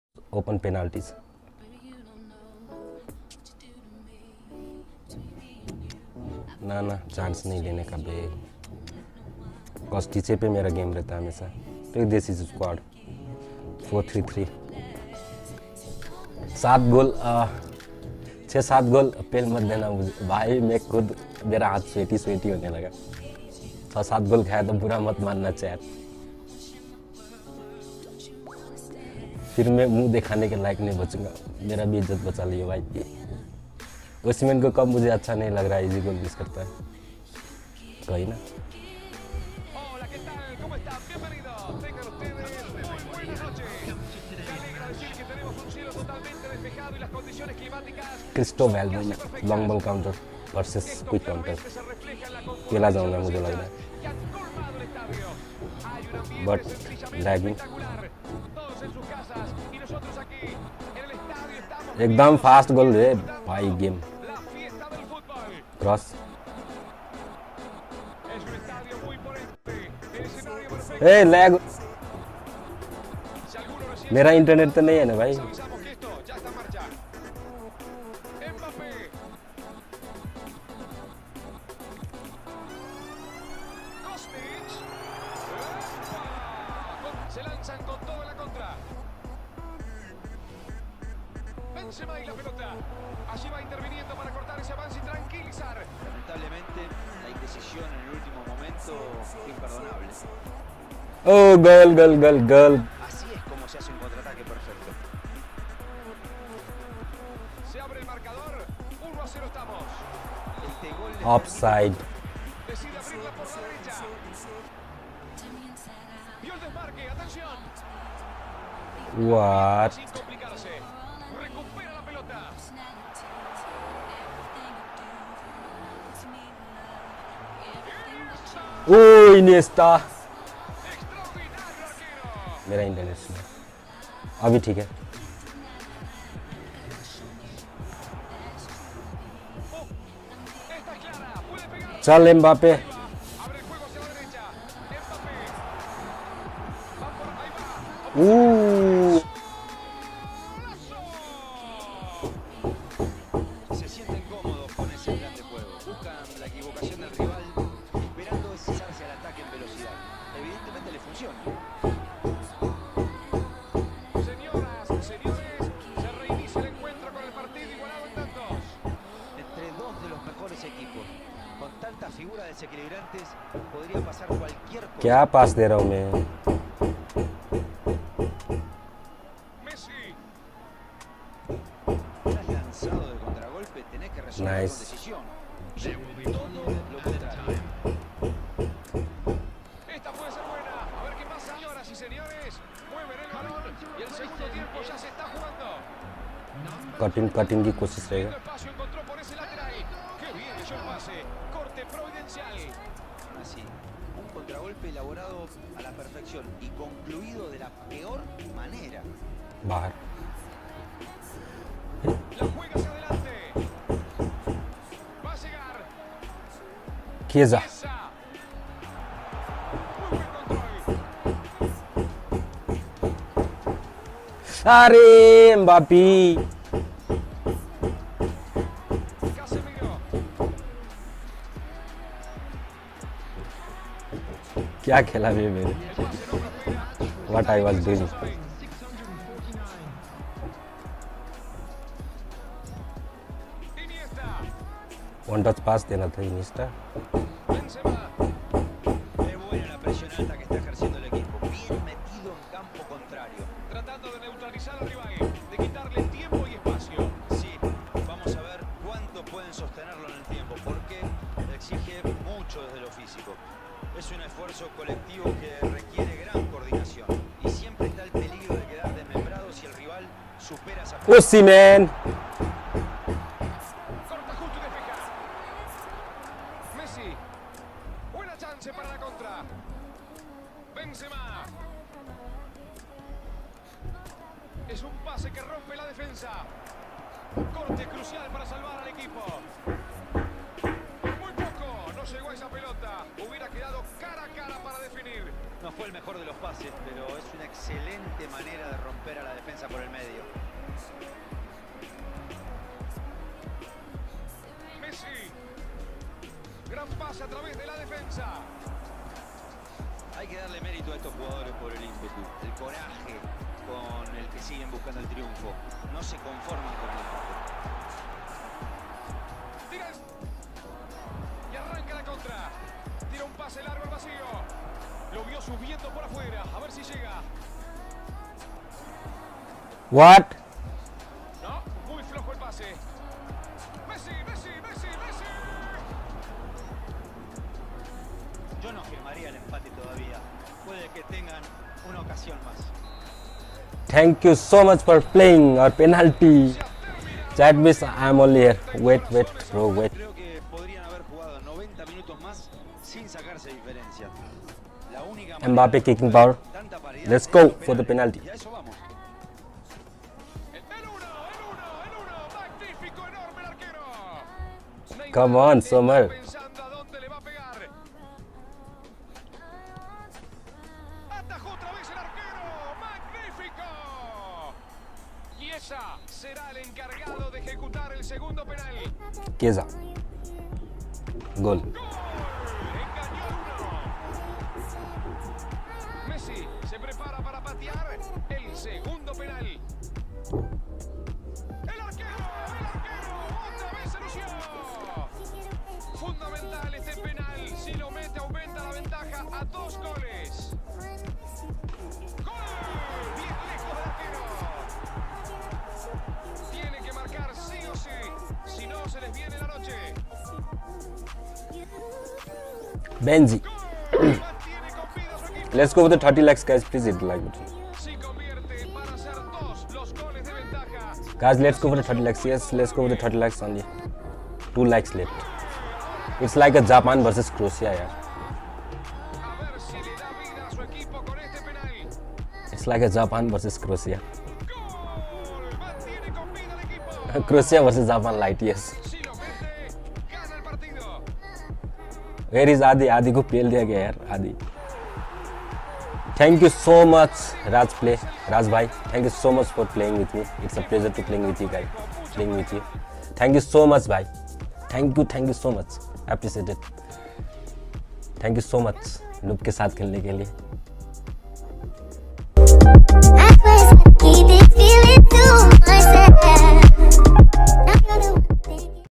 vs @TheSaiif (Quick Counter vs Long Ball Counter Gameplay) | FRIENDLY MATCH
We play a friendly a match its not a comparing video Enjoy the video And give me your feedback in the comment Did I keep uploading video like tjis on channel vs another streamer Please Comment kn that And Raj bhai is a op playee than me ♥Thanks for playing raj bhai with noob streamer Appreciated🔥
​ @RAJPlaysOP    vs  @LegendChhetriNO11   (Quick Counter vs Long Ball Counter Gameplay)  | FRIENDLY MATCH